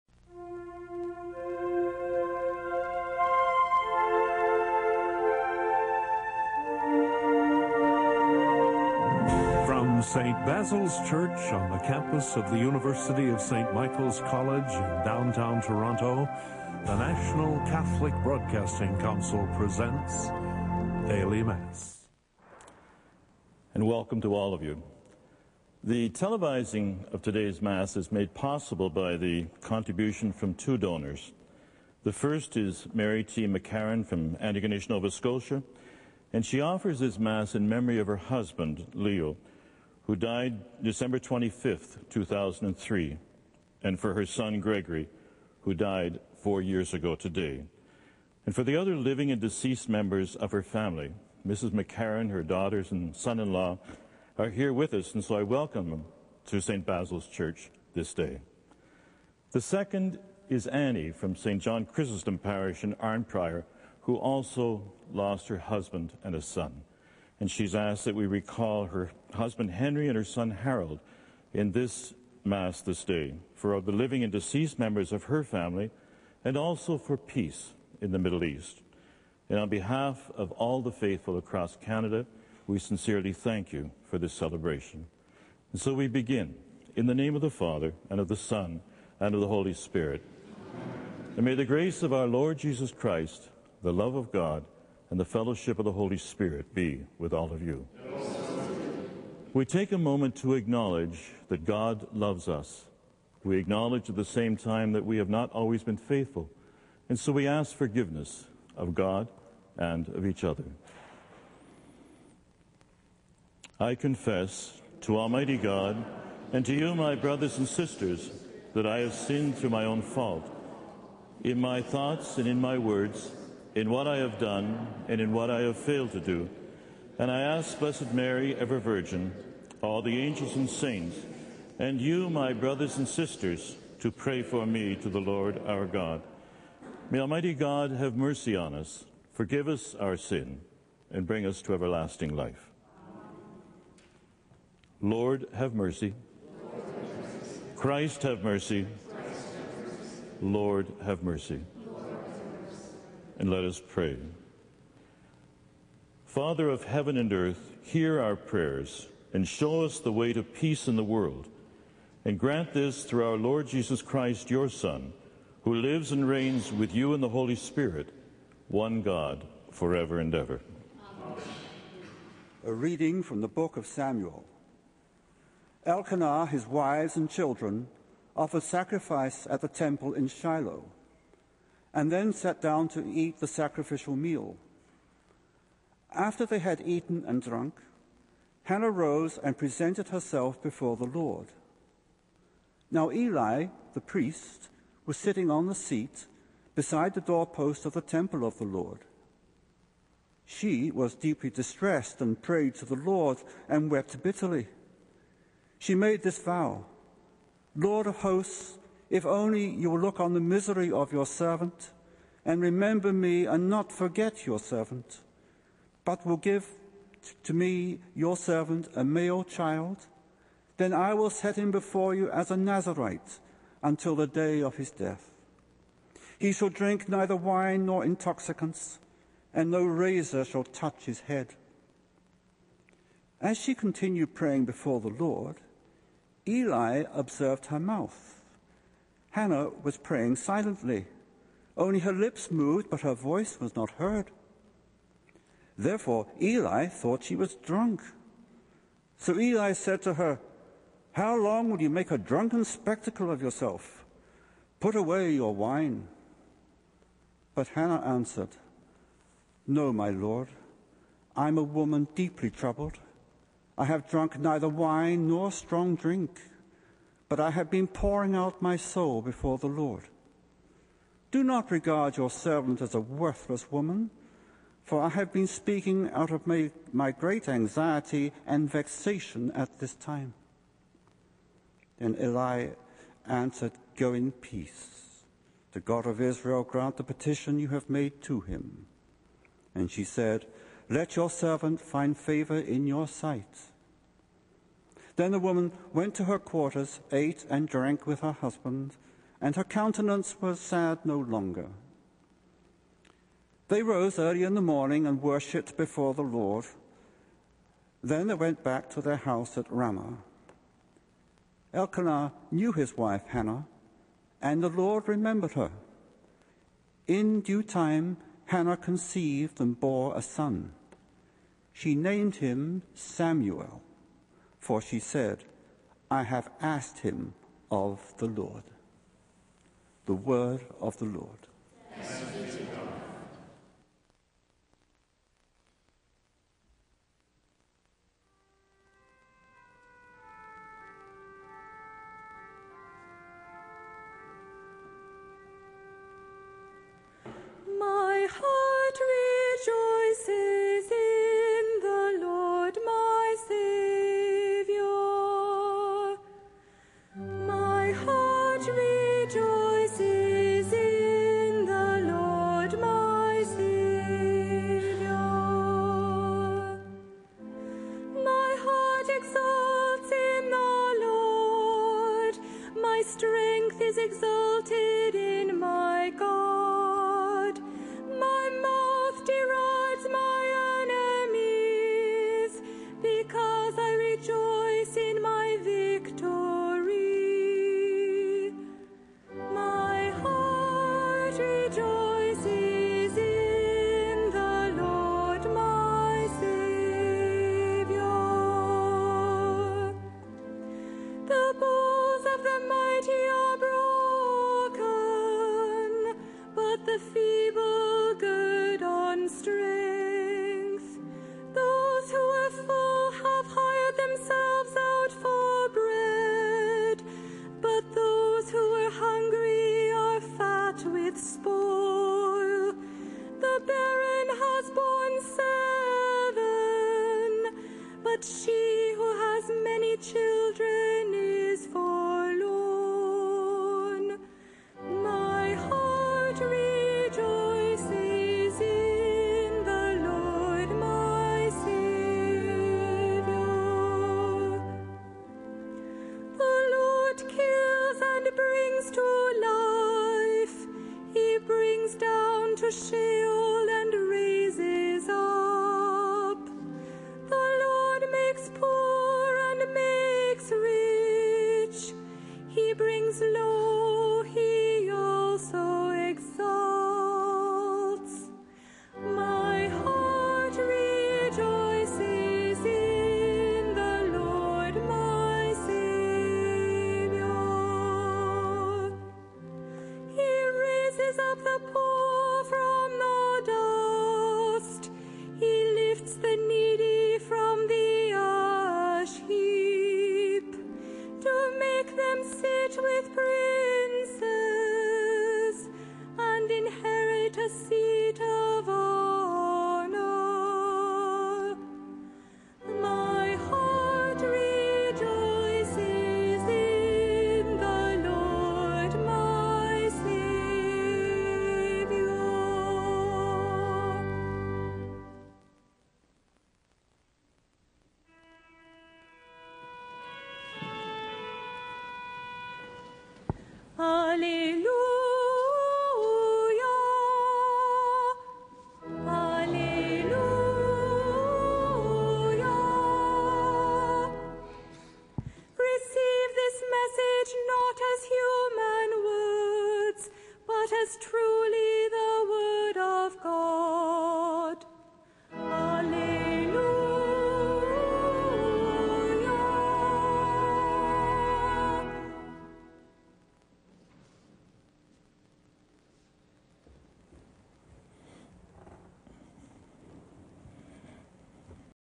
Daily Mass Part 1 of 3 Tuesday 15 January 2008
Fr. J. Lynch celebrates the entire Daily Mass in 3 parts, from the English-speaking Catholic Bishops of Canada and St. Basil's Church in Toronto.